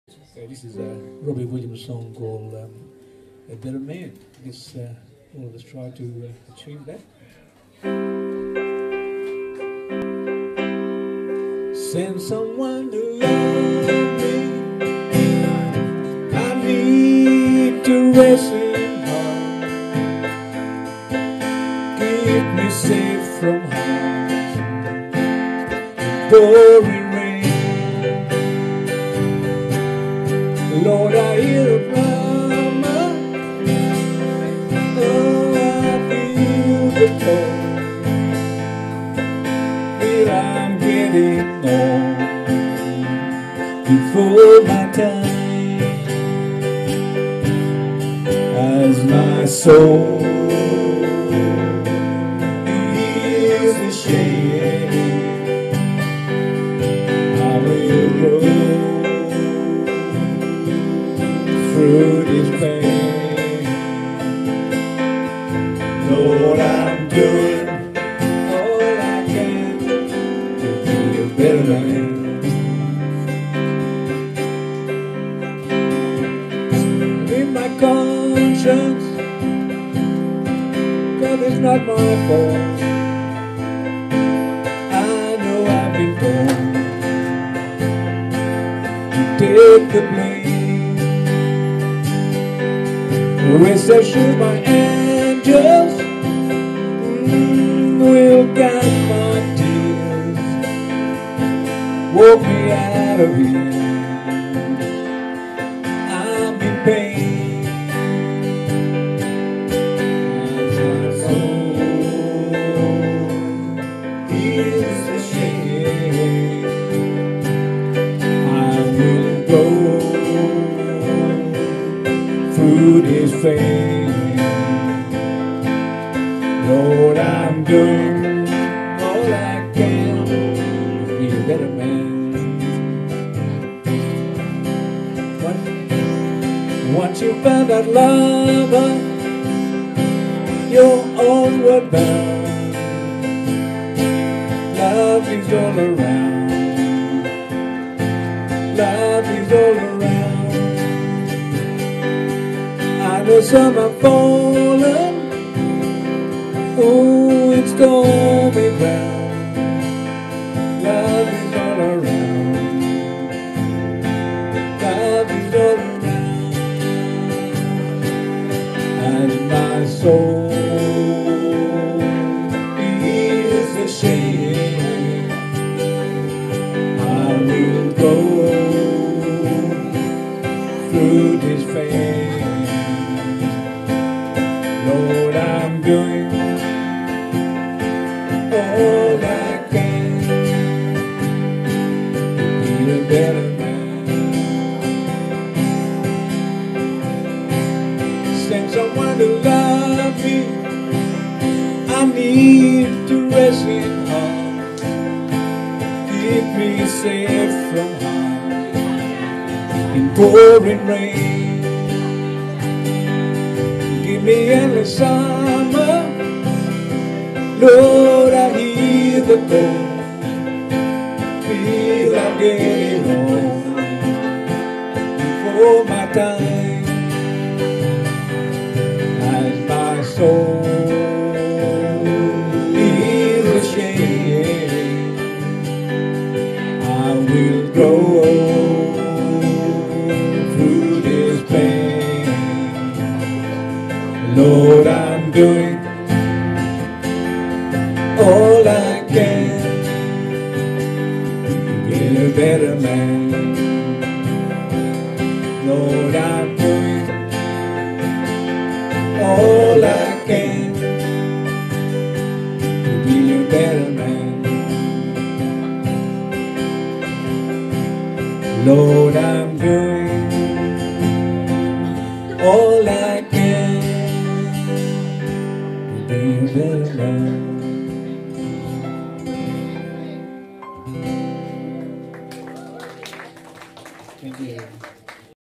Steve McMaughton & Ray Buttie at Club Picnic Point
Steve McMaughton & Ray Buttie playing at Club Picnic Point 17th June 2018